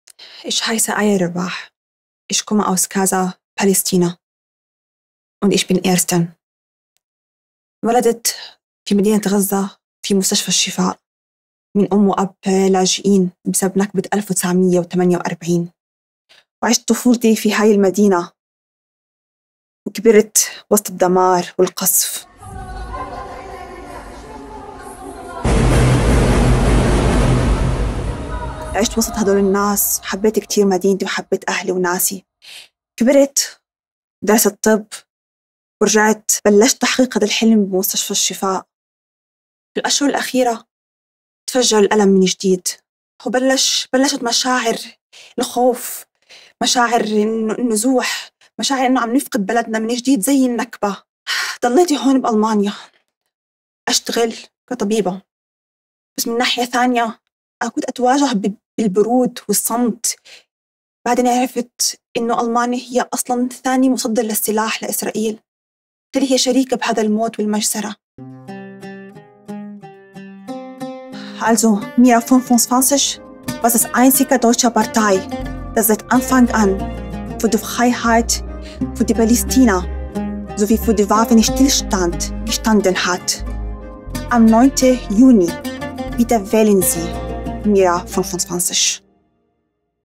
Nakba Day: Freedom for Palestine
May 15 marks 76 years of the Palestinian Nakba: a catastrophe - a tragedy of Palestinian dispossession, dehumanisation, death and suffering by the Israeli State supported by the U.K., US, Germany and other Western imperialists. More than 800,000 Palestinians were ethnically cleansed- brutally forced out of their indigenous homeland and made refugees for generations to come. We grieve for all Palestinian people - including those that have been mercilessly killed in the ongoing genocide in Gaza and continue to suffer under the brutal Israeli occupation.

Also today, at 16:10 CET our @MERA25_de TV Spot as part of our European Election Campaign will be aired on ZDF. Our spot will amplify not only our message, but the story of the Nakba - a story of Palestinian ethnic cleansing and dispossession that has been censored, twisted and erased from Western discourse.

We use this opportunity to tell the story of the Nakba to every single German watching ZDF today. To tell the truth about German complicity in the ethnic cleansing and the genocide of the Palestinian people. We centre the Palestinian narrative and voices in our campaign for Peace, Solidarity and Freedom. We centre the story of Palestinians represented by Ayah- whom the German media have failed, dehumanised and criminalised for 76 years.

On June 9, at the EU elections, let's make our governments pay at the ballot box. Vote for us in Greece, Germany and Italy (MERA25) so that we can call them out and unmask their hypocrisy.